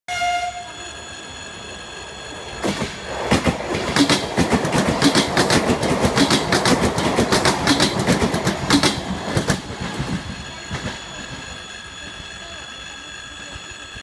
Электропоезд ЭШ2-010!
Электропоезд ЭШ2-010 был построен на заводе Штадлер Минск в г. Фаниполь в 2015 году по заказу ОАО "Аэроэкспресс", эксплуатируется с 2017 года. С 2020 года ходит на Киевском направлении МЖД по маршруту Киевский вокзал - аэропорт Внуково.

Видео снято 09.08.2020 у станции Переделкино.